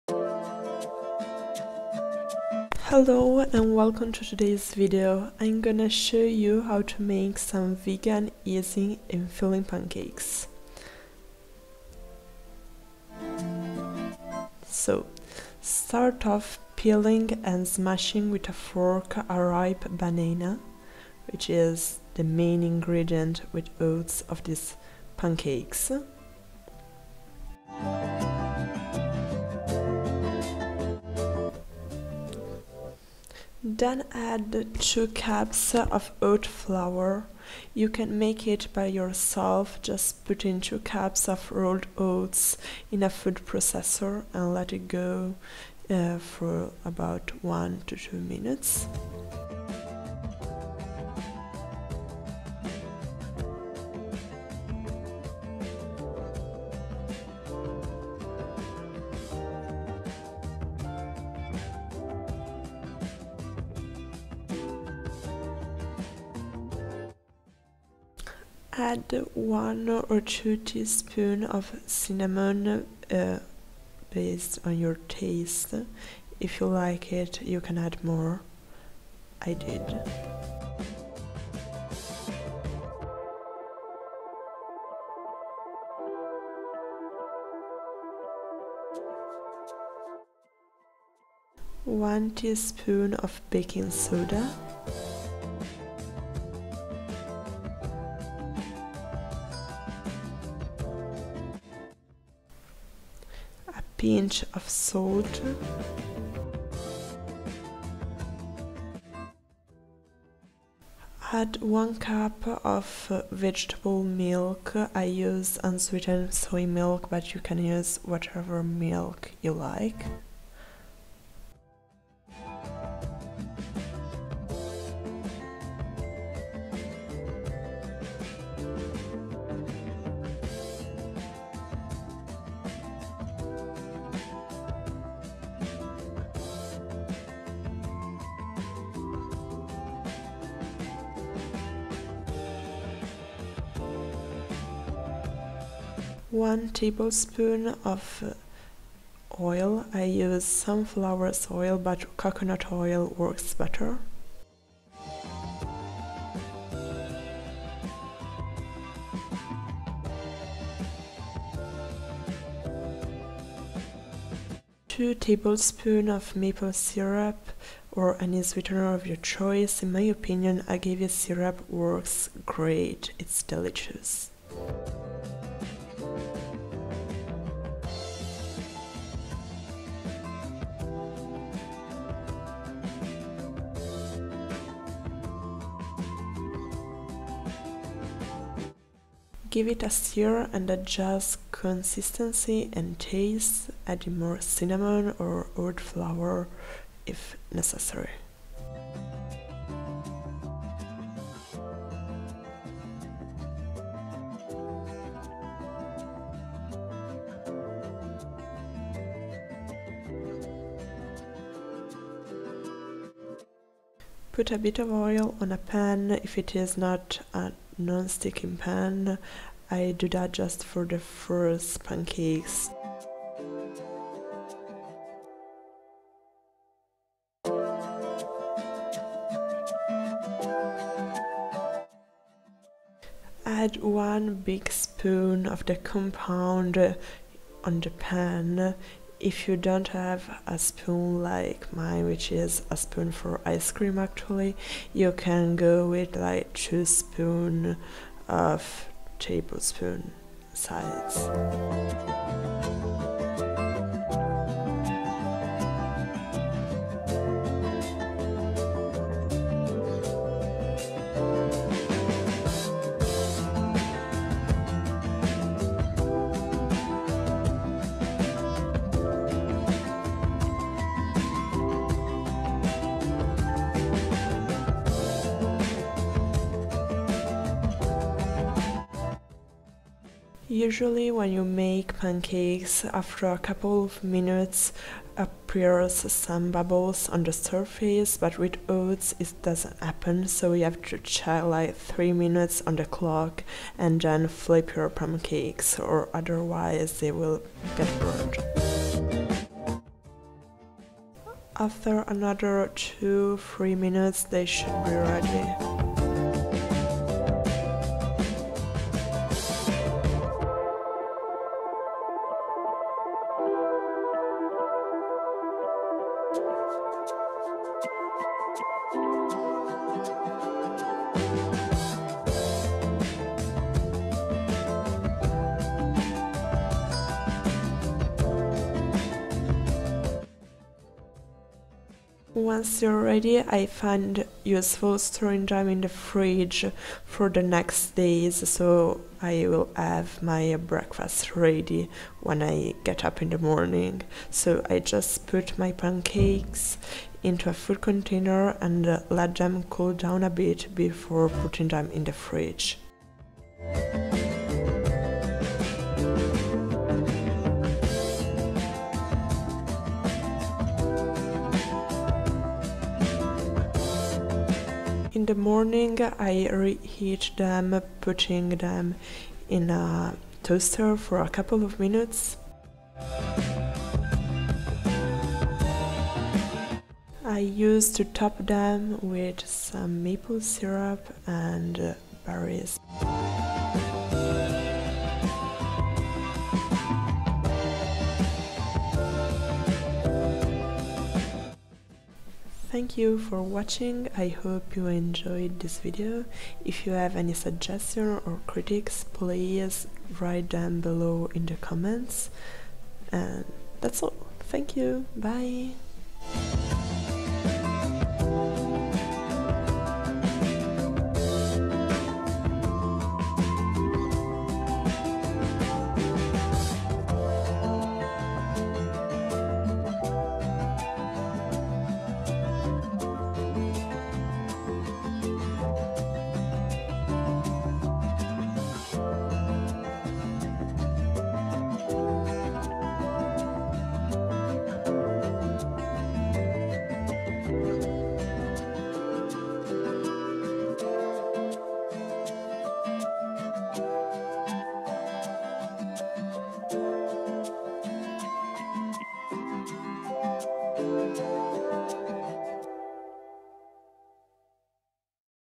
Easy Pancakes for a filling Breakfast // VEGAN
This video includes 1 easy, tasty, filling and vegan breakfast idea to start the day with a good mood.
Let me know if you try it!

Please, if you have suggestions/criticts write them below in the comment section.
This is my first YouTube video so every tip is very useful for me!

Sorry if the english is not perfet: I am actually Italian.

INGREDIENTS

2 cups of oat flour*
1 cup of vegetable milk
1 ripe banana
1 tsp of baking soda
2 tsp of cinnamon (or more :) )
1 tbsp of oil
1 tbsp of maple syrup (or any sweetener of your choice)

*you can make it by blending 2 cups of rolled-oats with your food processor/mixer

Anna